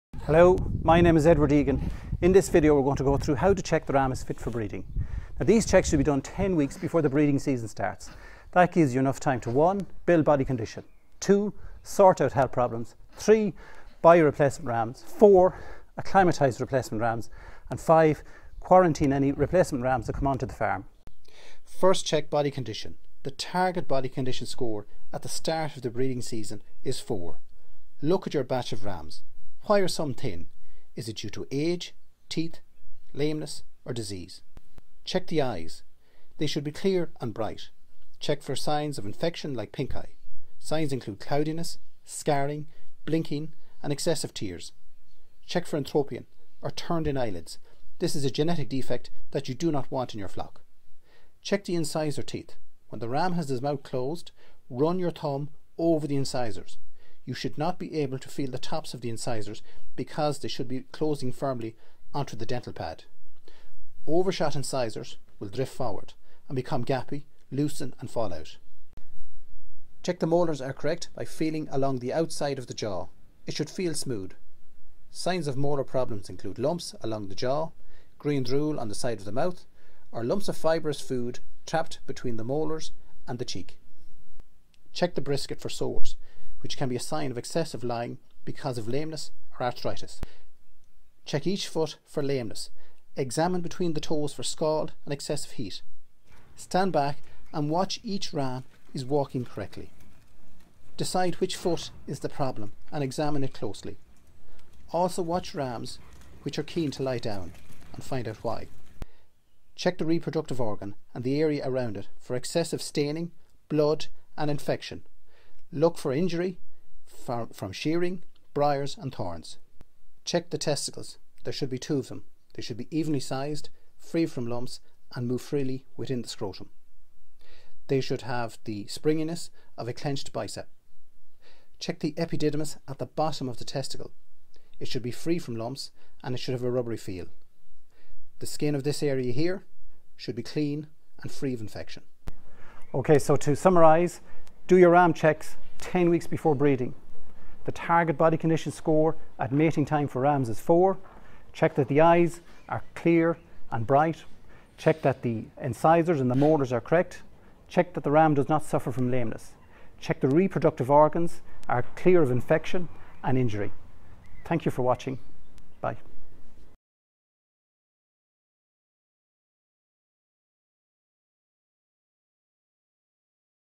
How to check the ram is fit for breeding - Virtual Sheep Week
Breeding checks on rams ideally should be done 10 weeks before the breeding season begins.  That gives you enough time to build body condition, sort out any health problems, buy replacement rams, acclimatise replacements or quarantine replacements. In this short video, Edward Egan, Teagasc drystock advisor talks through how to check rams are fit for breeding.